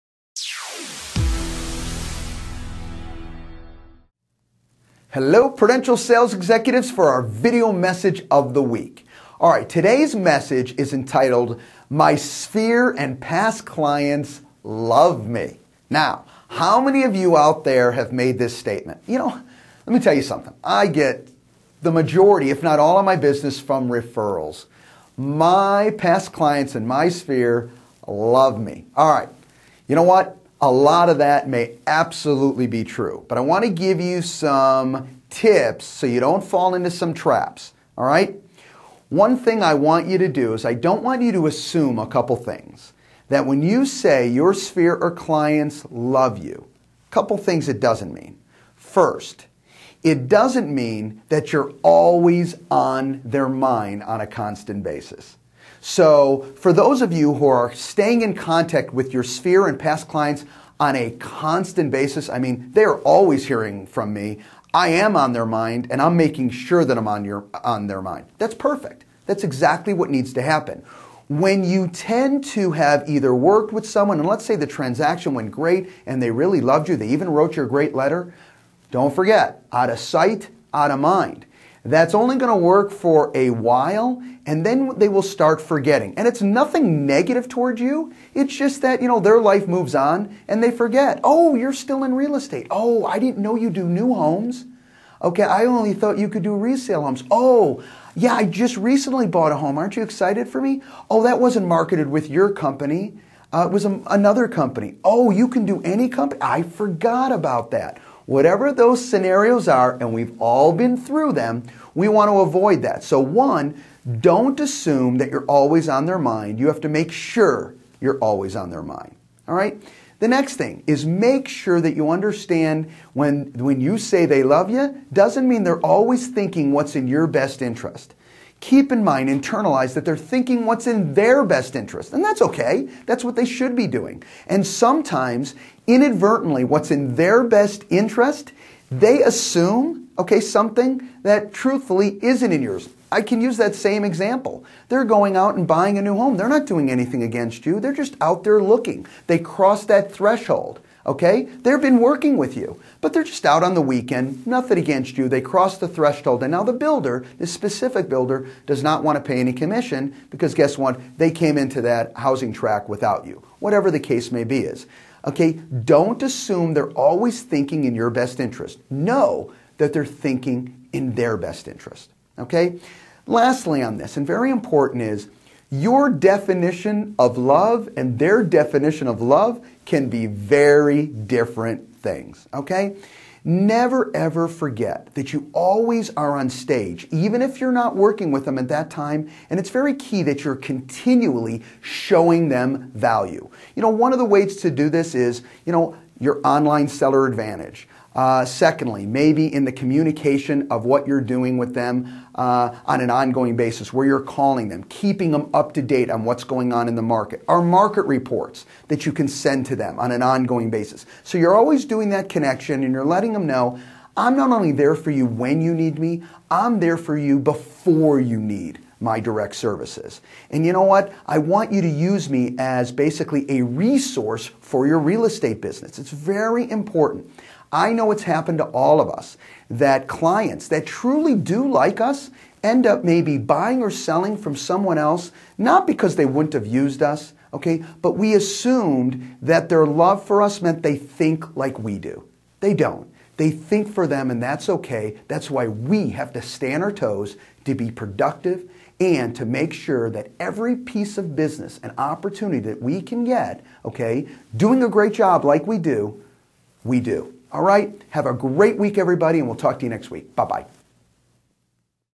Real Estate Training- My Sphere and Past Clients Love Me- 22
My Sphere and Past Clients Love Me

•Do not confuse love with --

o  Always on my mind
o  I will do what is in your best interest
o  Their definition of Love